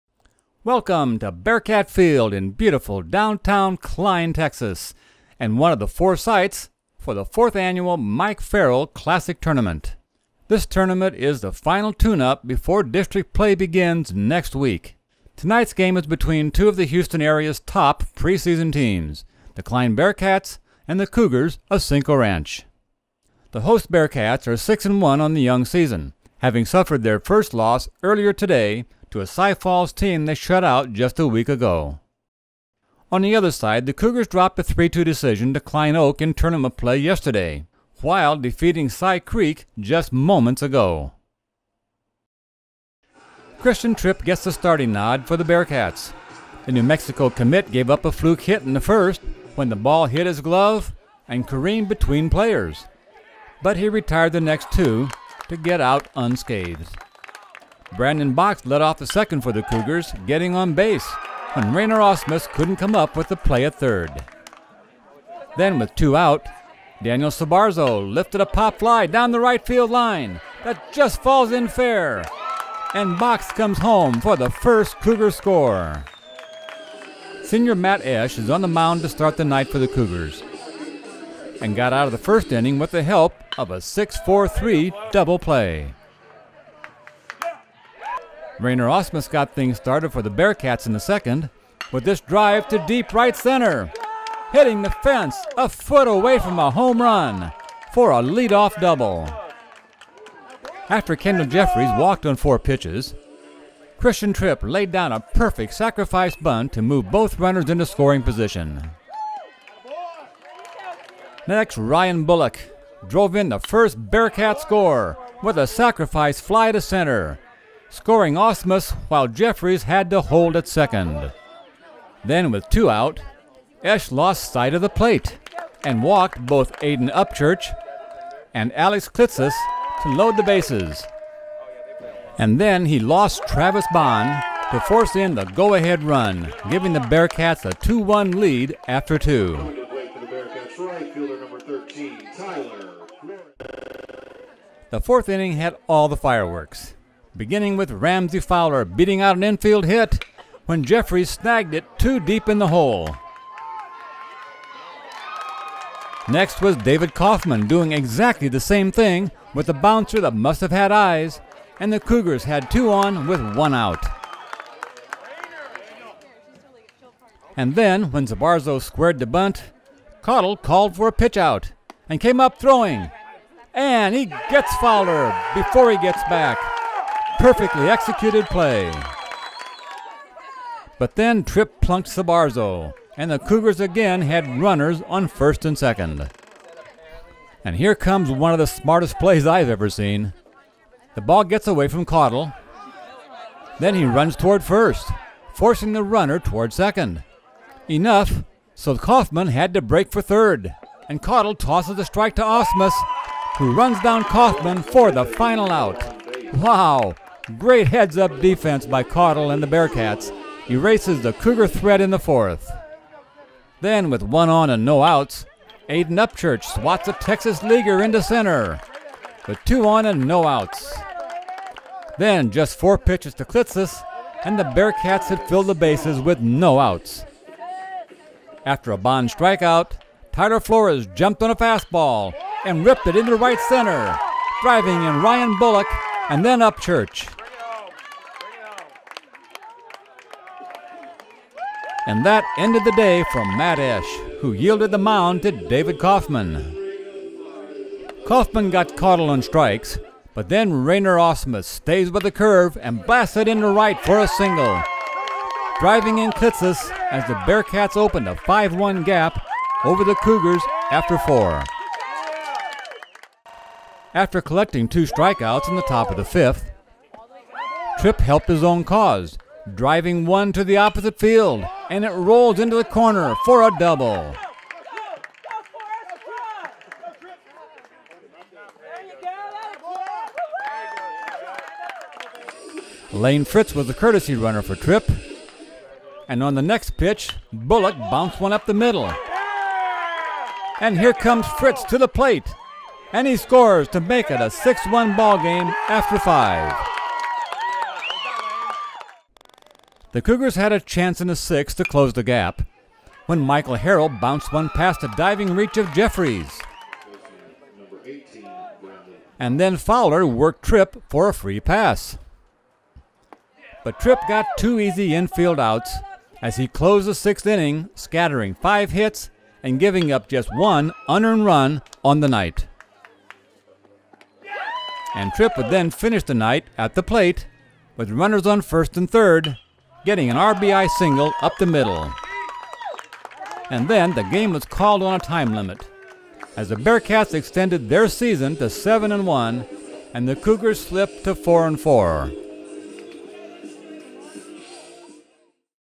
Cinco Ranch vs. Klein - Texas HS Baseball 2014
In the final tournament tune up before district play begins the Cinco Ranch Cougars take on the Klein Bearkats in a Mike Farrell tournament game at Bearkat Field.